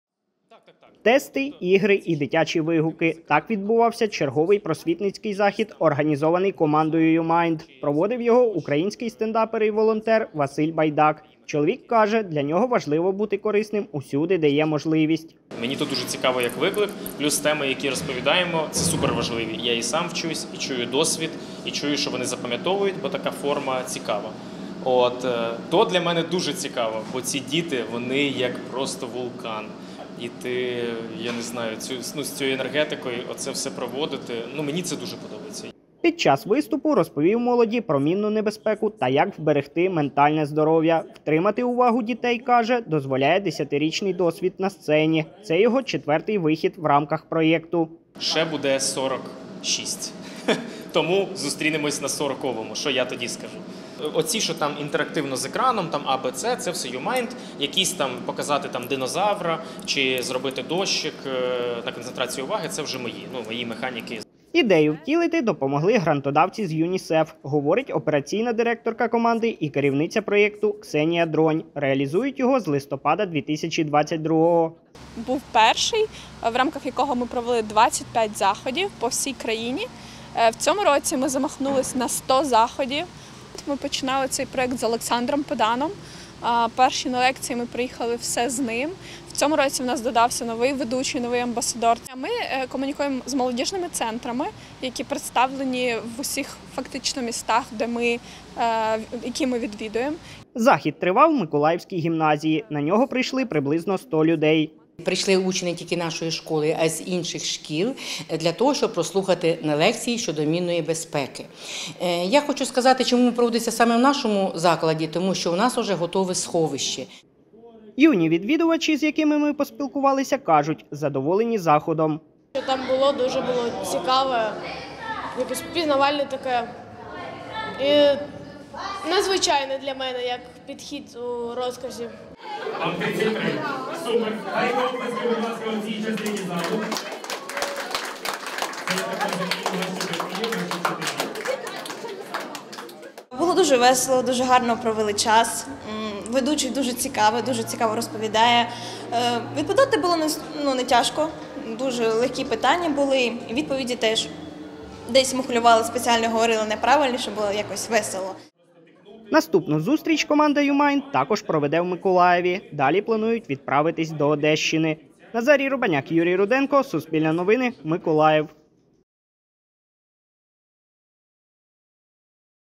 "НеЛекції". У Миколаєві відбувся просвітницький захід для школярів
В одній з миколаївських гімназій 28 липня відбувся захід, на якому стендапер Василь Байдак розповів молоді про мінну безпеку, важливість освіти та здоров'я. Реалізуються відповідні зустрічі за кошти Unicef в рамках проєкту "НеЛекції", розповіла його керівниця Ксенія Дронь.